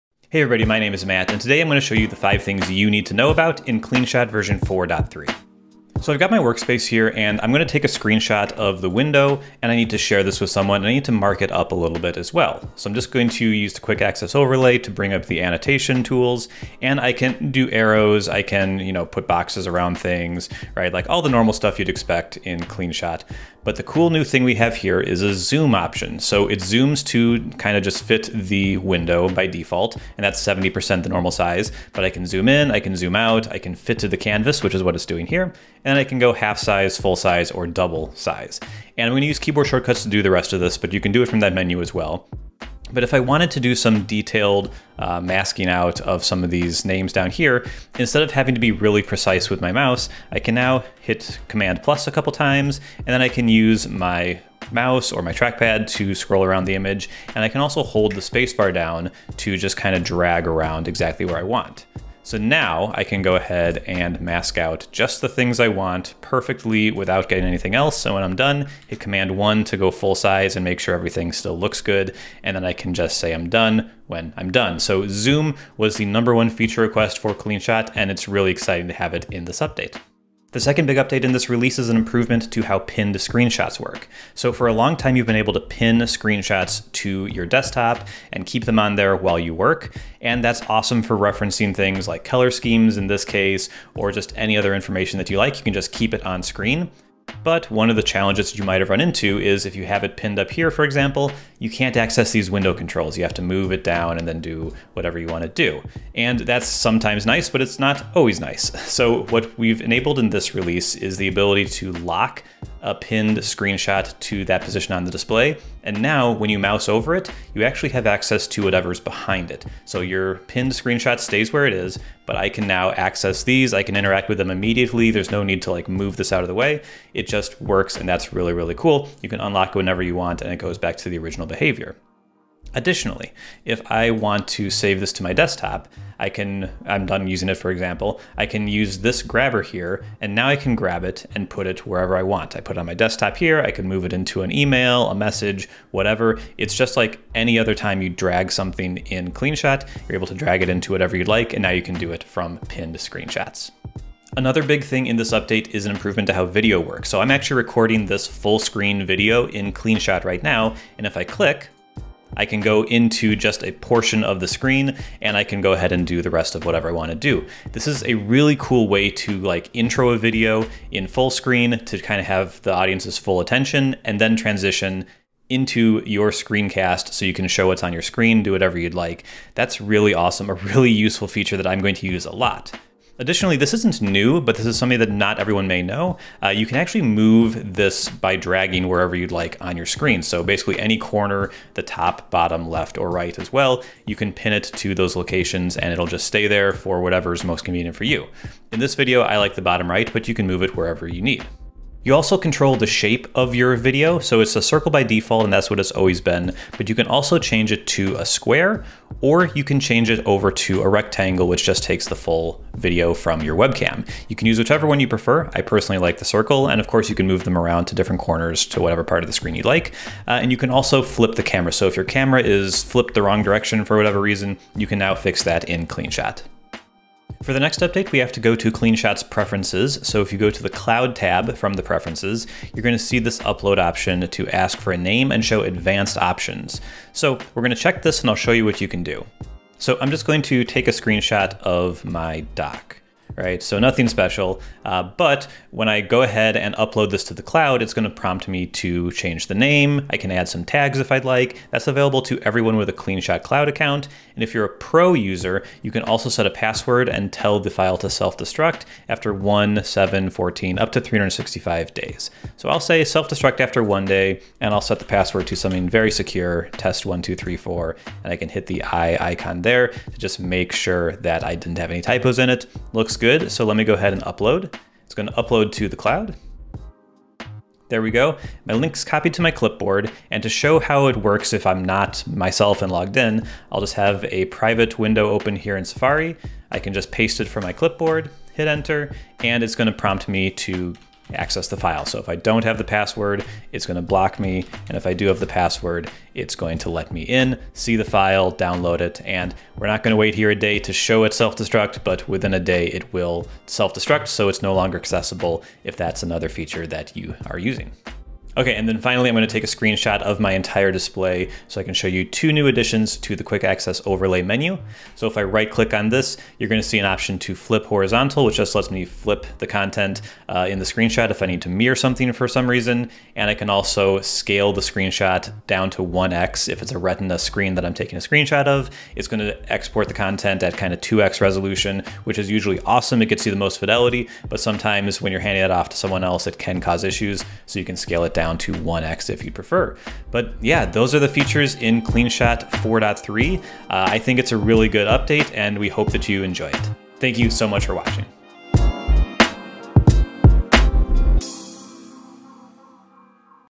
CleanShot 4.3 - Canvas Zoom, Lock Mode, New Camera Options, Password Protection
With the latest update, we’re introducing some new features that we think you’re going to love.

- 🔍 Added option to zoom the canvas in annotation tool
- 🔒 New "Lock mode" for pinned screenshots
- 🖐️ Added "Drag me" button to pinned screenshots
- 🧑‍💻 You can now click on camera overlay to make it full screen
- 📺 Added square and rectangle camera shape options
- 🌩️ Set password protection and expiration time before upload (Pro)
- Changed behavior of "Caputure Area & ..." shortcuts - they won't ignore "After Capture" settings anymore
- Added "Capture Area & Save" keyboard shortcut
- Added option to set keyboard shortcut for Pin action in annotation tool
- Added "Scale Retina to 1x" to Quick Access Overlay context menu (right click)
- Added "Flip Horizontally" to Quick Access Overlay context menu (right click)
- Improved Spotlight tool by smoothing rounded corners
- Improved interactions with Spotlight and Pixelate objects
- Improved camera size settings by scaling it proportionaly to recording area
- Sublte change of app icon
- Fixed bugs with detecting links in the OCR tool and added option to disable this feature
- Fixed bug with copying Retina screenshots to clipboard from Annotate
- Minor bug fixes and UX improvements

Chapters:
0:00 Zoom canvas
1:20 Lock pinned screenshots
2:12 Drag me
2:33 Camera overlay improvements
3:46 Password protection
5:08 New quick actions
5:39 Ending